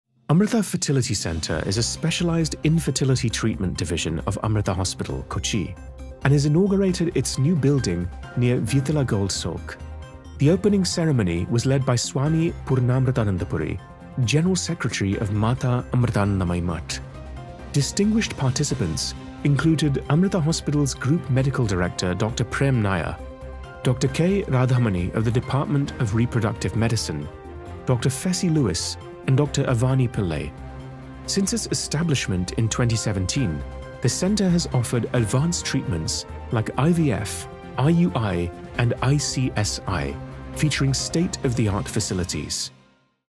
Amrita Fertility Center Launches New Facility to Enhance Treatment
Amrita Fertility Center is a specialized infertility treatment division of Amrita Hospital, Kochi @amritahospitals and has inaugurated its new building near Vyttila Gold Souk. The opening ceremony was led by Swami Purnamritananda Puri, General Secretary of Mata Amritanandamayi Math. Distinguished participants included AmritaHospitals Group Medical Director Dr. Prem Nair, Dr. K. Radhamani of the Department of ReproductiveMedicine, Dr. Fessie Lewis, and Dr. Avani Pillai. Since its establishment in 2017, the center has offered advanced treatments like IVF, IUI, and ICSI, featuring state-of-the-art facilities.